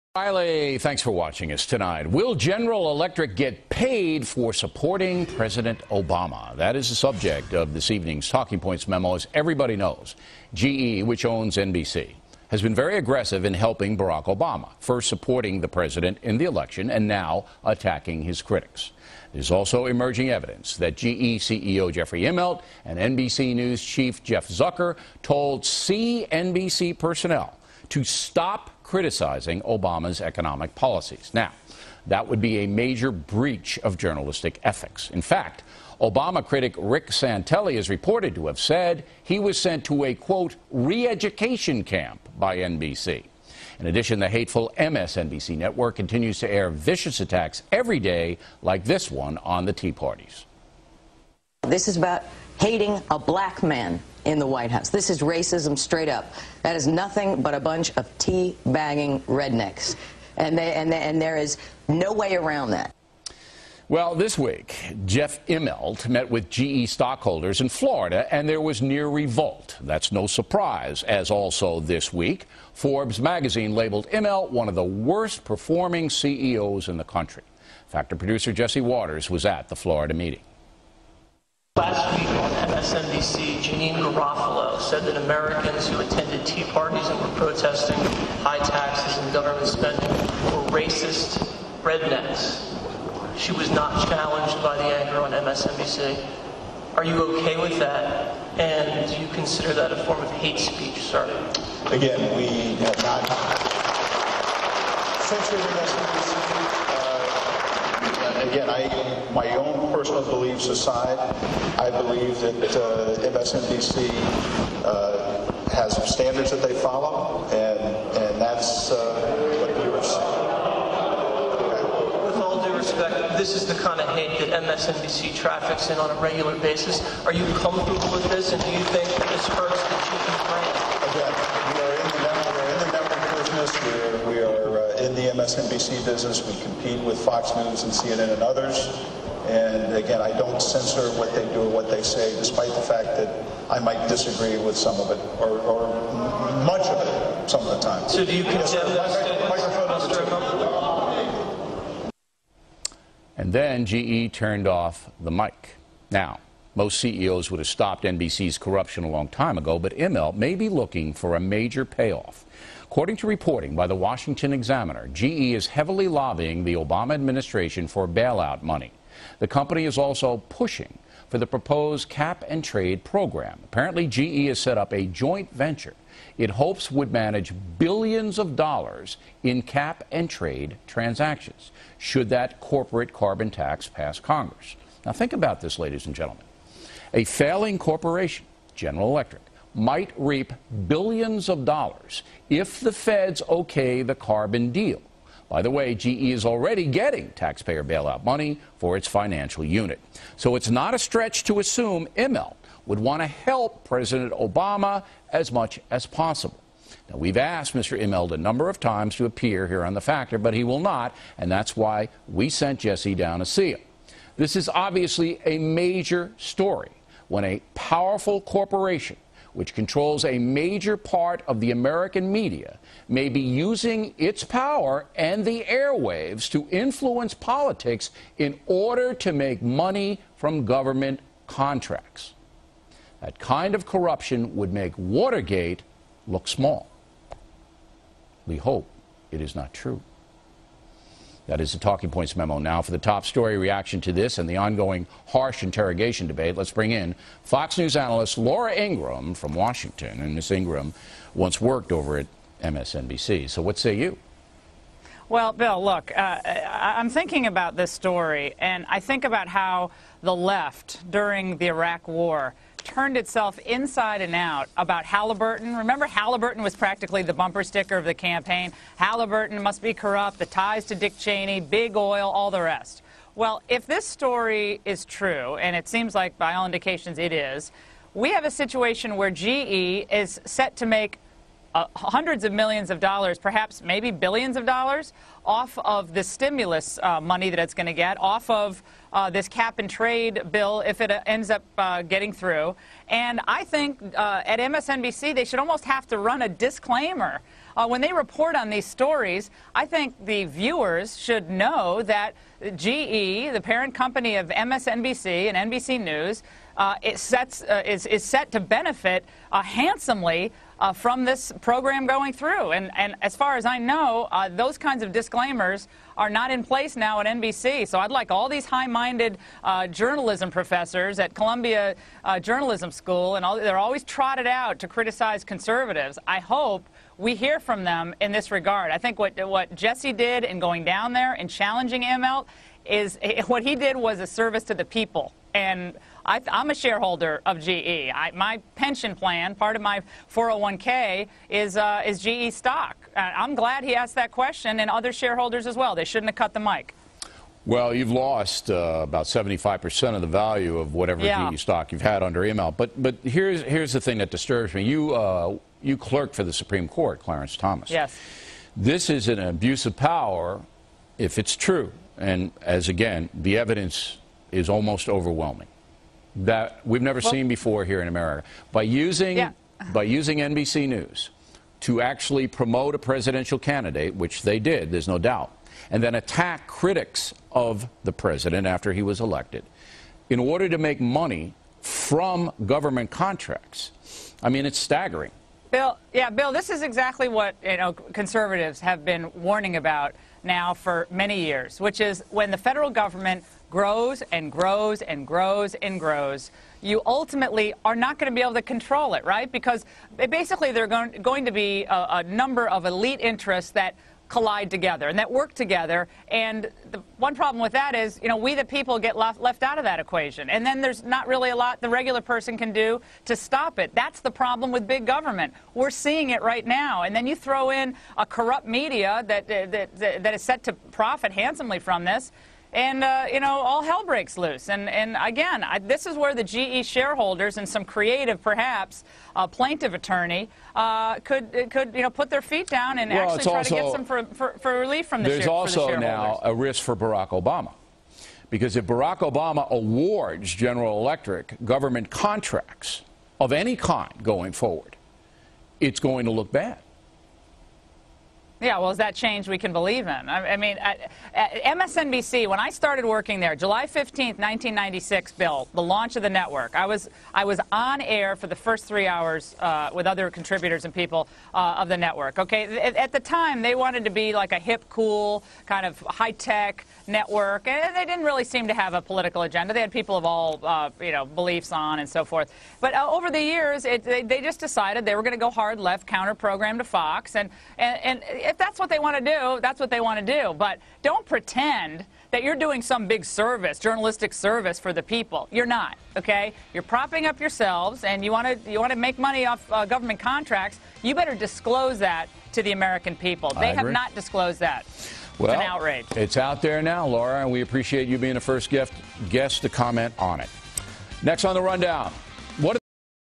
MSNBC Aids Obama for $$$, Official Policy! GE Wants Gov Cap n Trade Biz! O'Reilly & Ingraham
Santelli Sent to GE pro-Obama Re-Education Camp!!